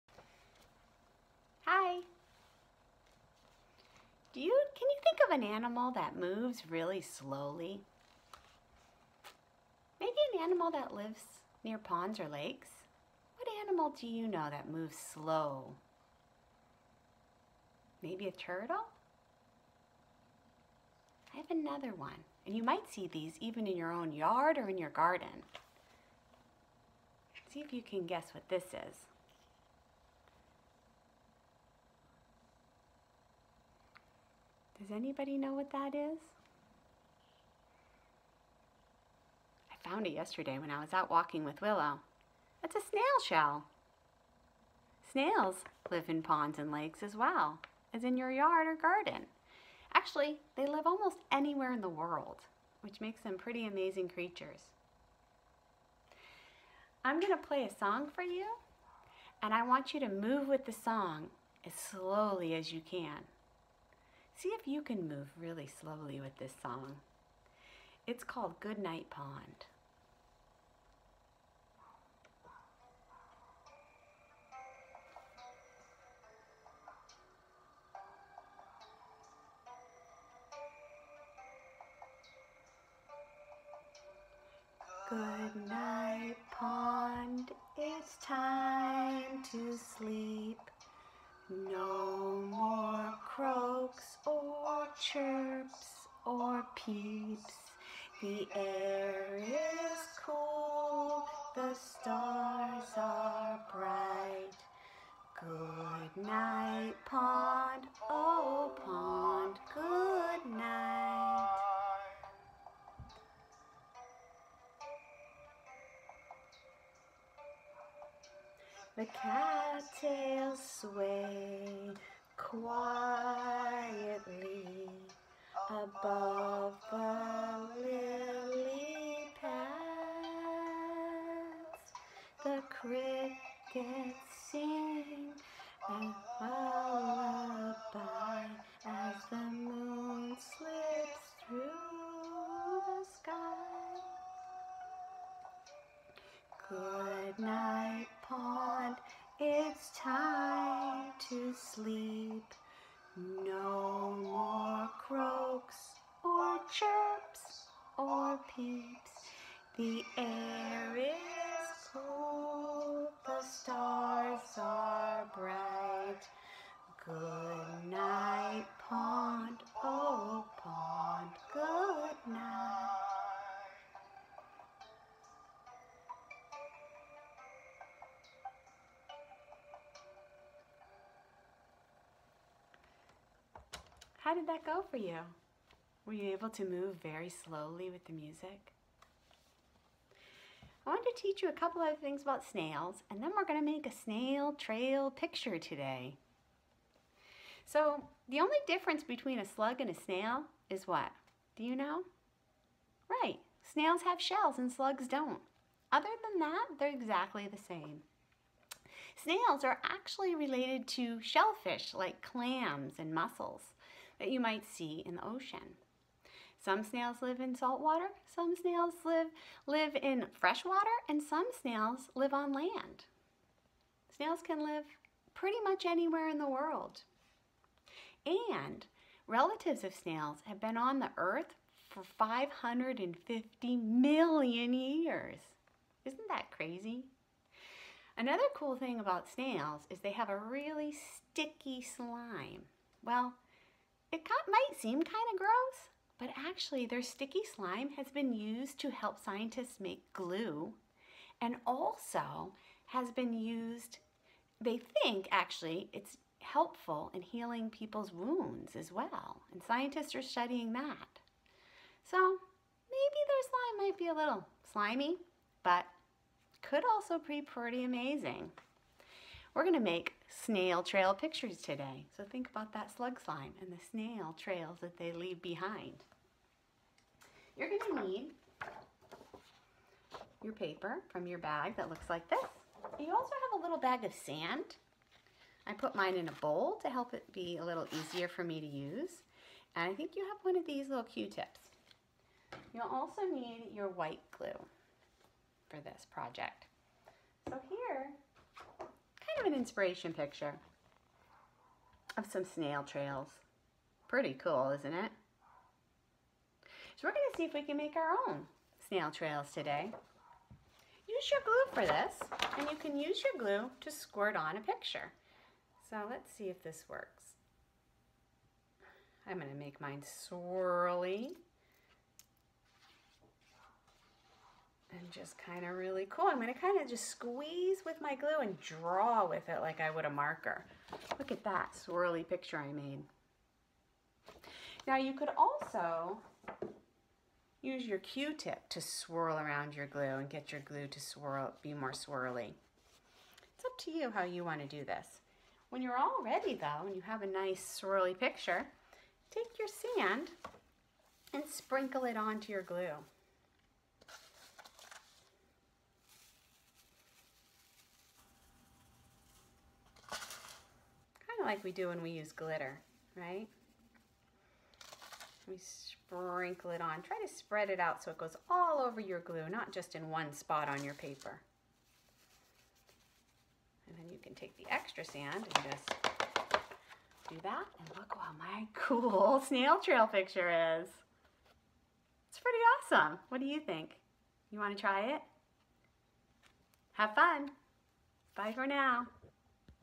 Friday April 10th
Today we'll practice moving like a snail, learn about snails and make a snail trail picture!